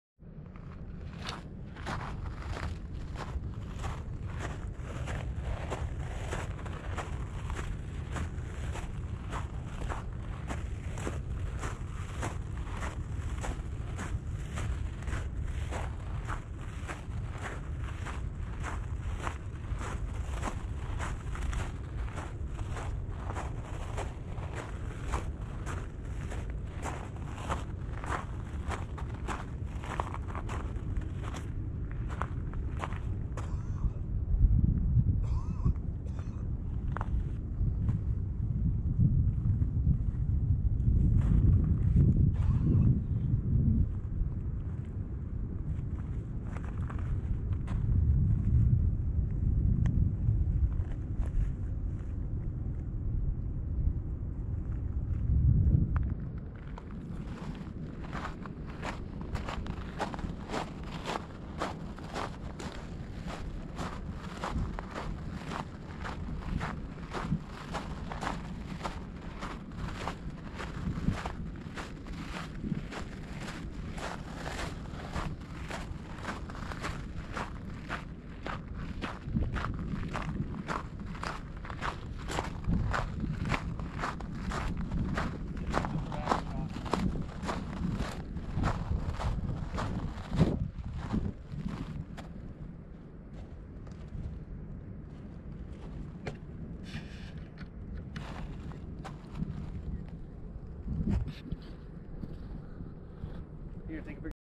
Iceland winter into sunset
January 2019 pit stop during the southern coastline drive. Had a great shot of the sunset, clouds finally parted and revealed some light. Iceland, a beautifully desolate place. Suzuki Jimny, the only vehicle for the job.